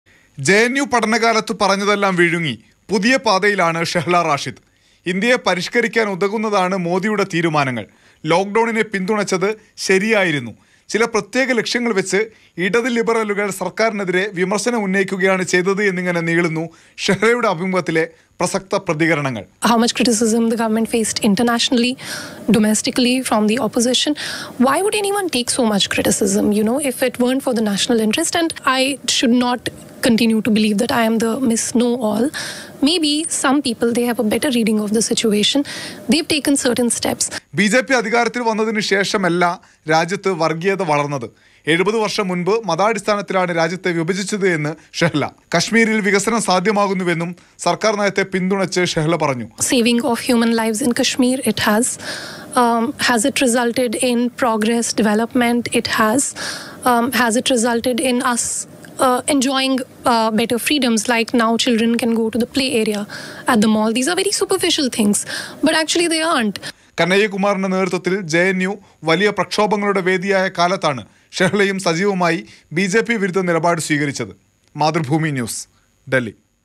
പ്രധാനമന്ത്രി ഇന്ത്യയെ പുരോ​ഗതിയിലേക്ക് നയിക്കുന്നു- BJPയെ പിന്തുണച്ച് JNU മുൻ വിദ്യാർഥി നേതാവ്
Happy viewing!